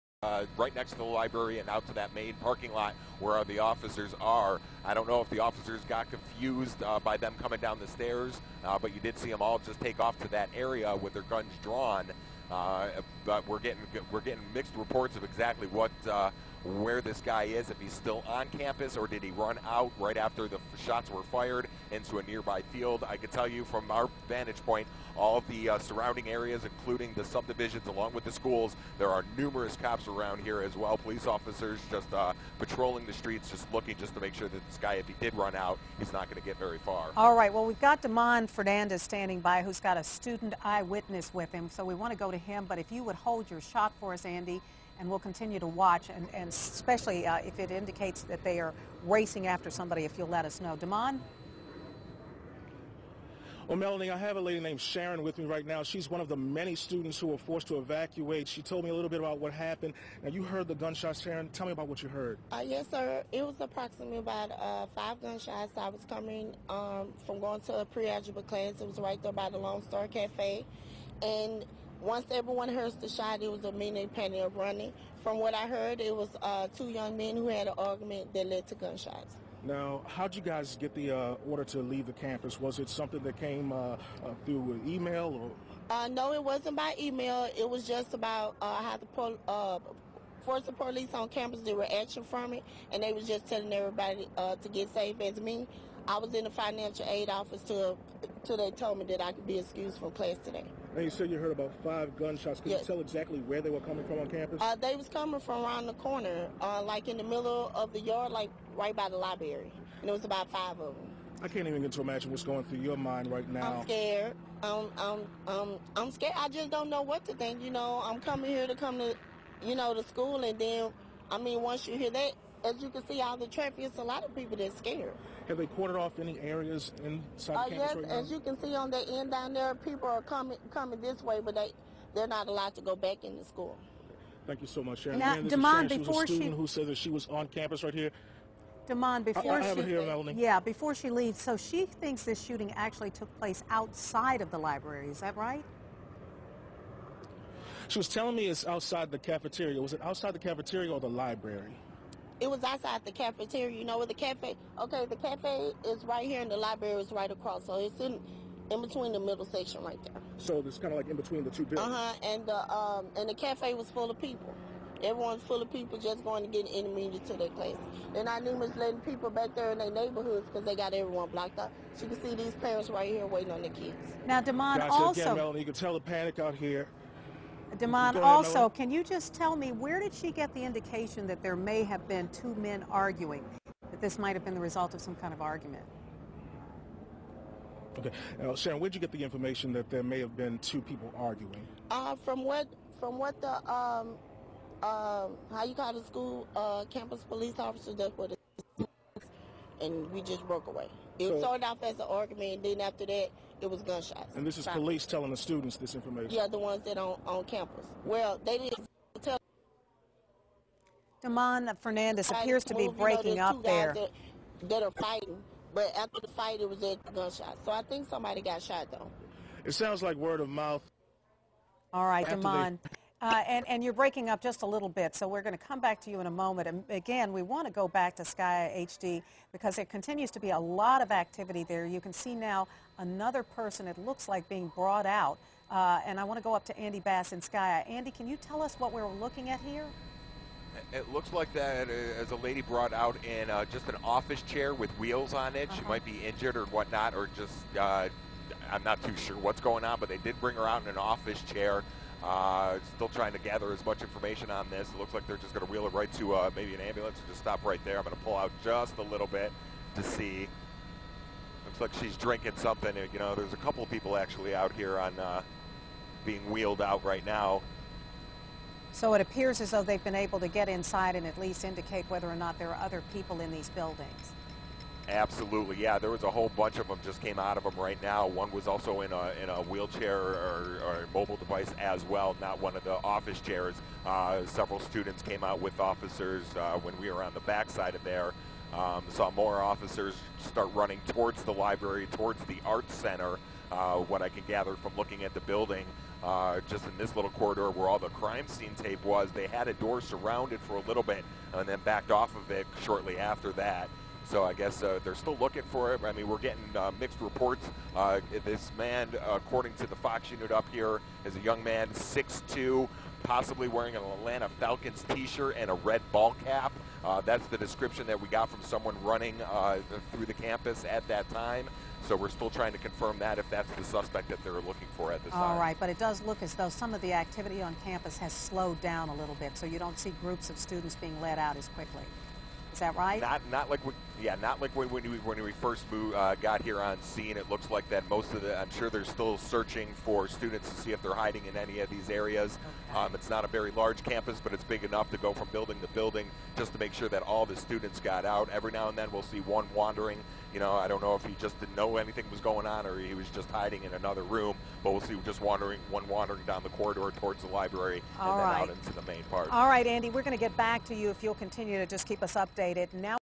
3 shot at Houston-area college
Sheriff's official: 3 people wounded in Texas college shooting, person of interest in custody